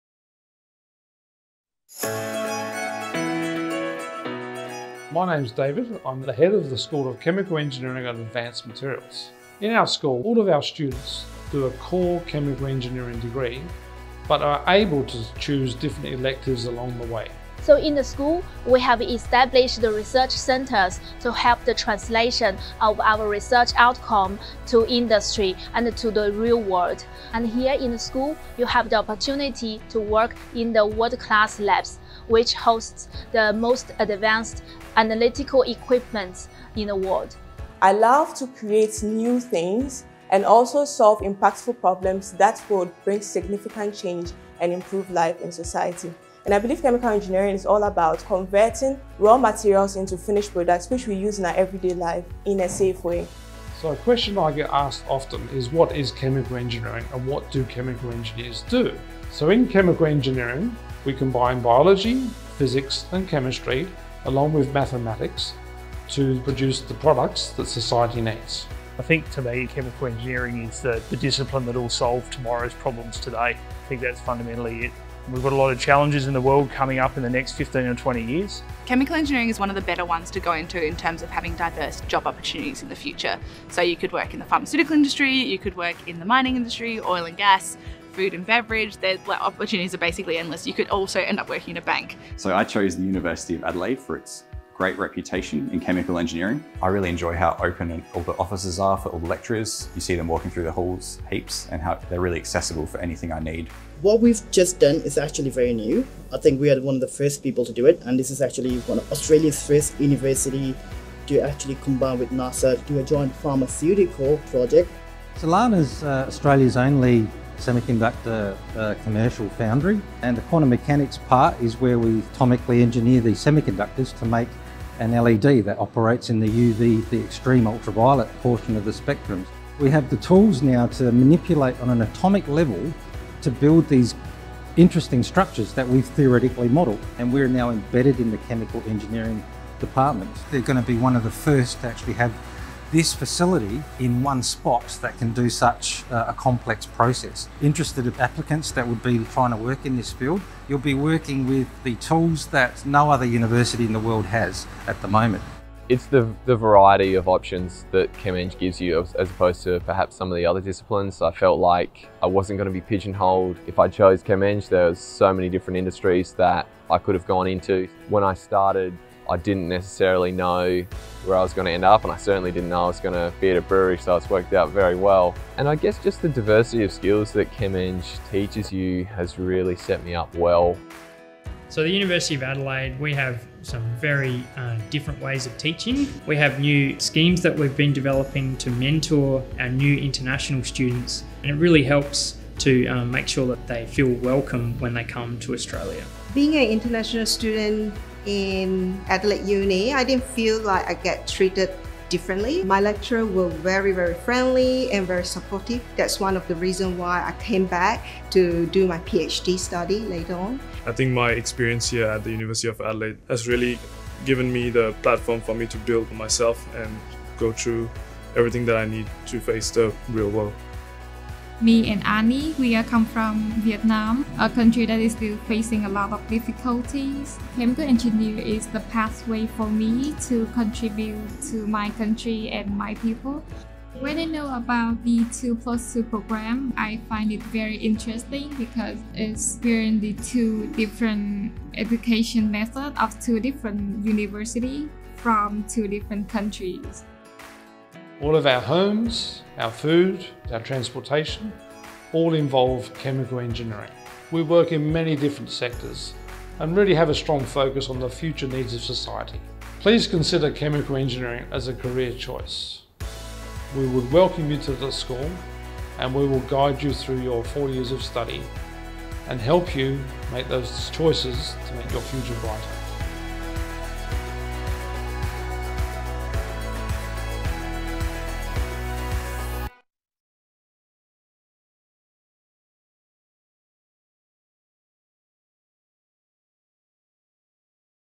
The School of Chemical Engineering and Advanced Materials
At the School of Chemical Engineering and Advanced Materials in the Faculty of Engineering, Computer and Mathematical Sciences, our expert services are applied to areas like: minerals, pharmaceuticals, food, cosmetics and chemical engineering. Our work is industry leading, and assists with solving important global challenges.